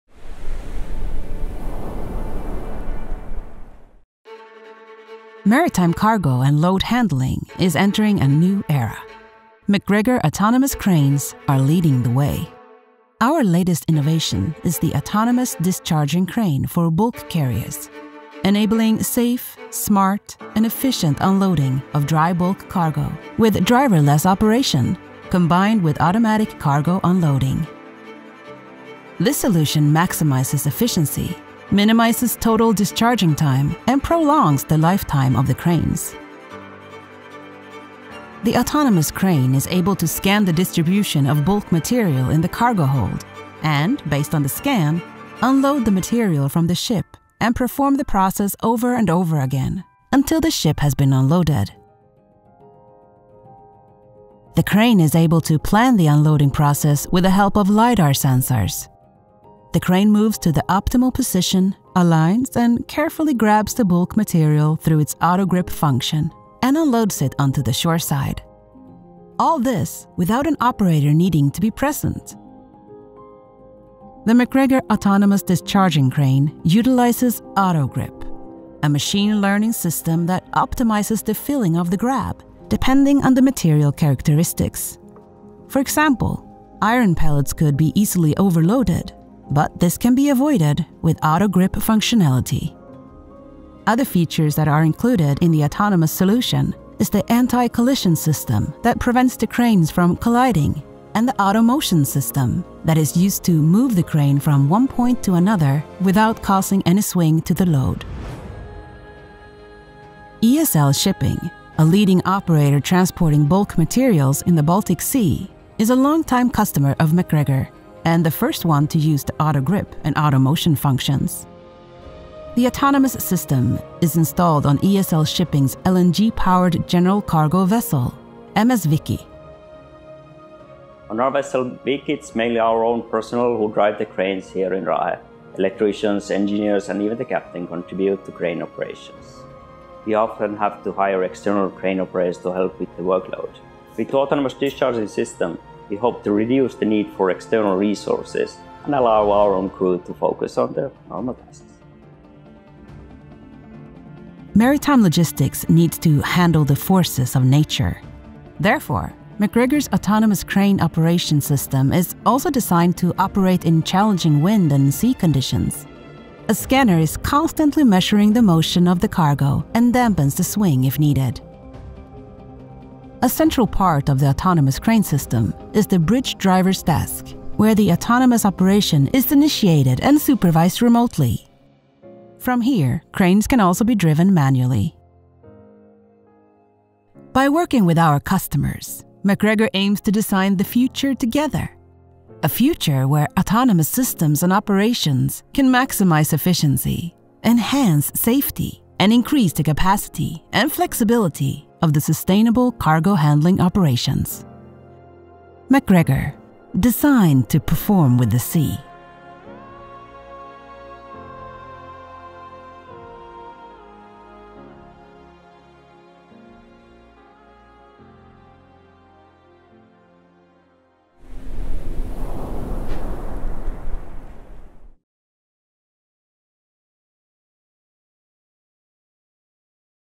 Pioneering Highly Automated Crane Operations with ESL Shipping
Our new video is created in collaboration with ESL shipping. It showcases the practical benefits and real-world applications of MacGregor highly automated cranes.